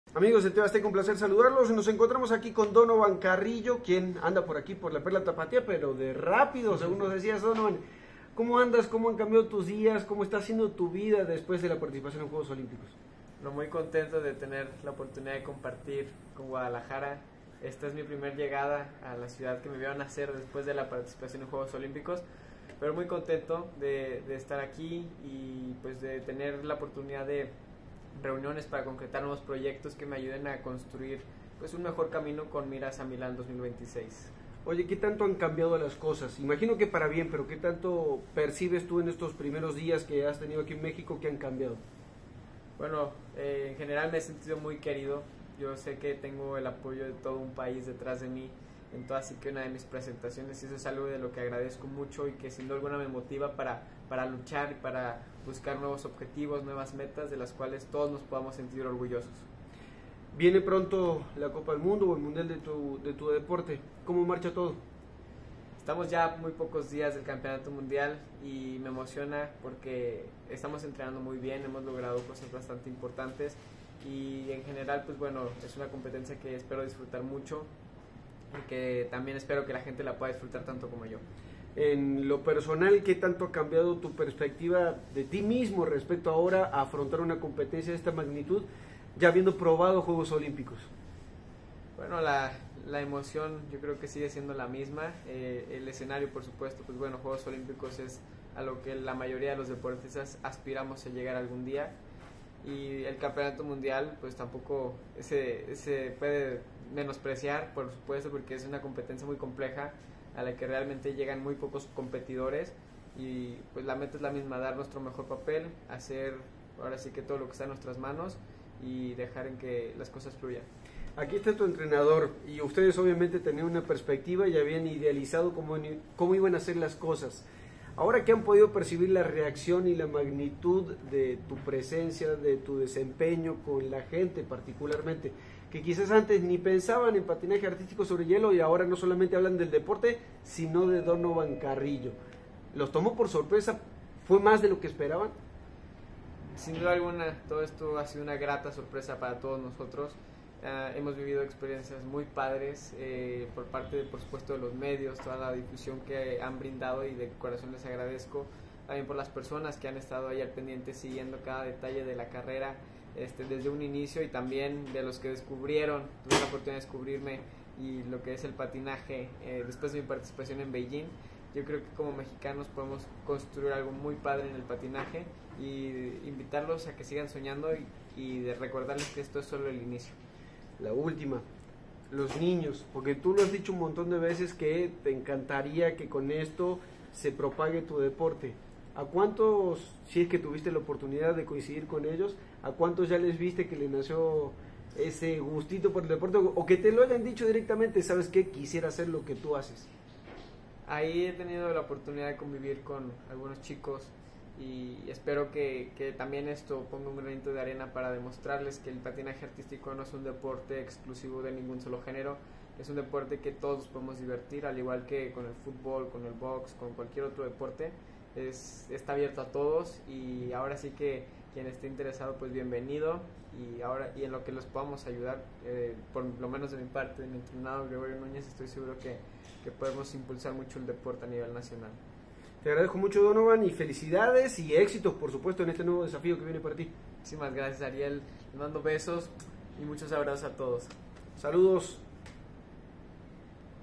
Entrevista con Donovan Carrillo | El mexicano prepara sorpresas para sus próximas competencias
Exclusiva | Donovan Carrillo reveló cuánto ha cambiado su vida después de la participación en los Juegos Olímpicos de Bejijng2022. 🙌

El partinador jalisciense aseguró que desea impulsar el patinaje a nivel nacional. ⛸️

🎙 Ariel Leguizamón